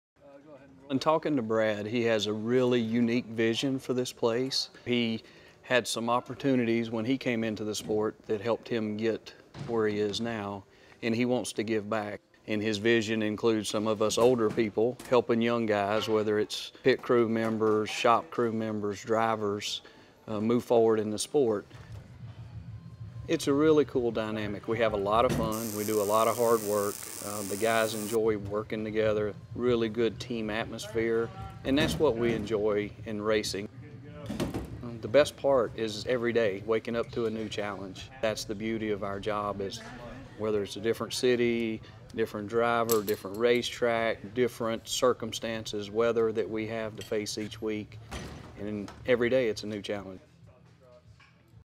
ION Cequent™ EXTRA: Doug Randolph on Working for BKR
Doug Randolph talks about what it's like to work for Brad Keselowski and BKR.

Cequent™ Connecting You With Your Life